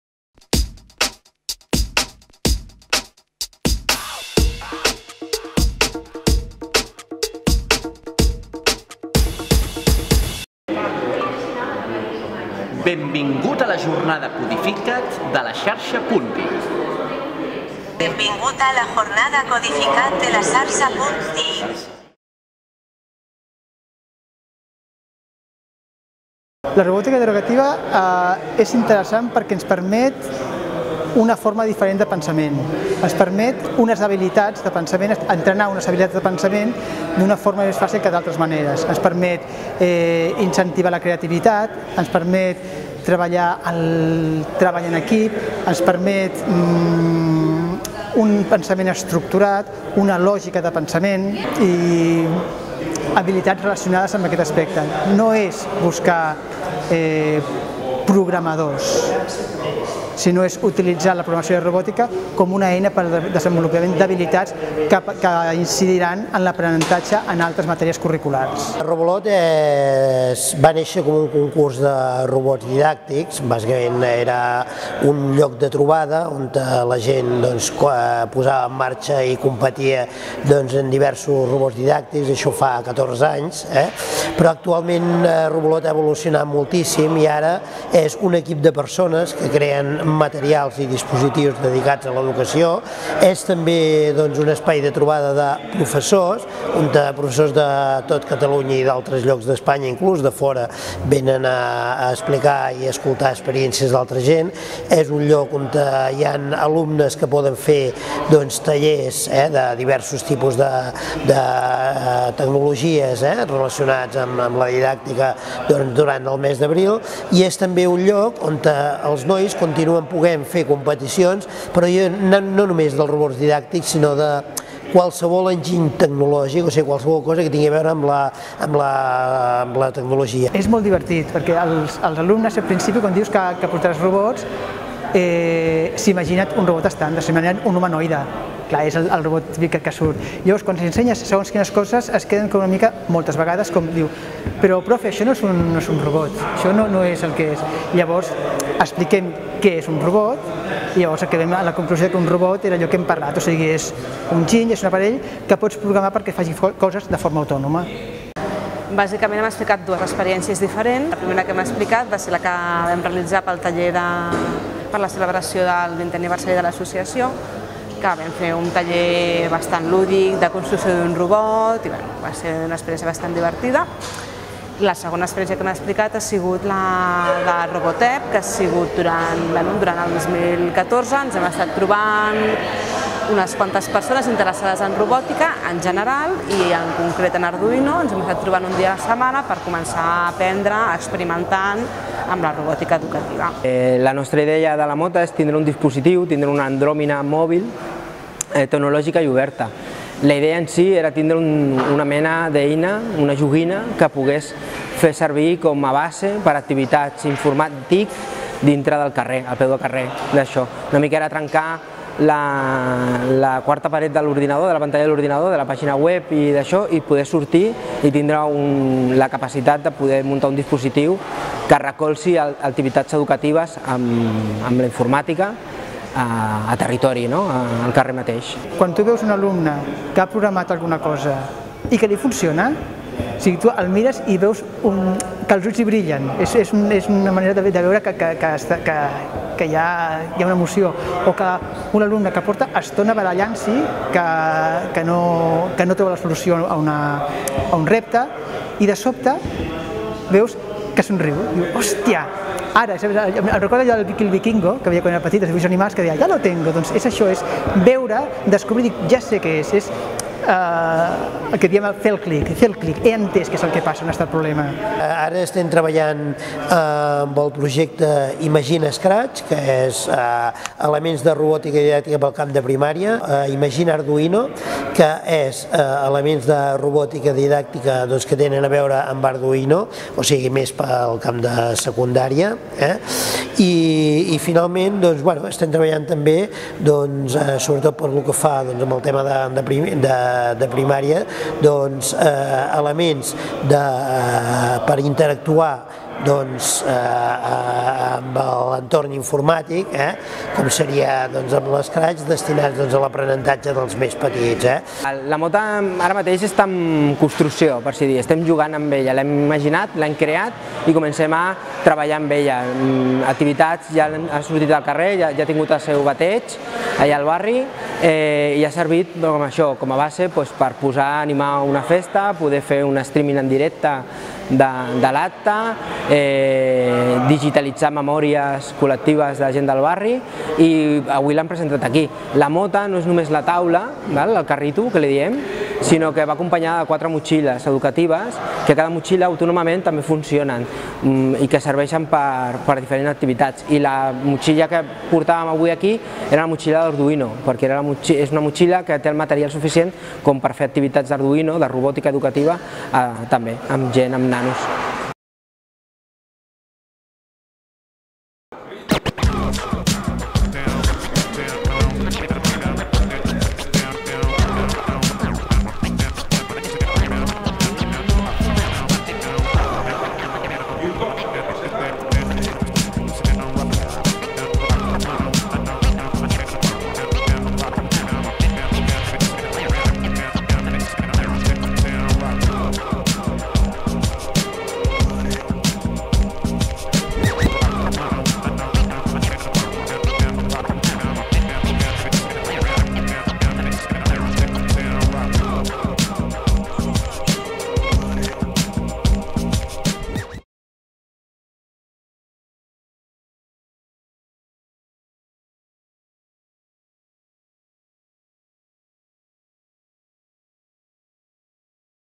Jornada Codifica't a Olot - Xarxa Punt TIC
Una vintena de persones van conèixer diferents iniciatives relacionades amb la robòtica a la jornada Codifica't, que va tenir lloc el 15 d'octubre a l'espai L'Ideal d' Olot. També van tenir un primer contacte amb la programació als dos tallers pràctics que s'hi van portar a terme.

Organitzada per la Xarxa Punt TIC amb la col·laboració del Consell Comarcal de la Garrotxa, la jornada s'emmarcava en un esdeveniment que ha tingut lloc de l'11 al 17 d'octubre a nivell europeu per potenciar la programació i la robòtica, l'European Code Week.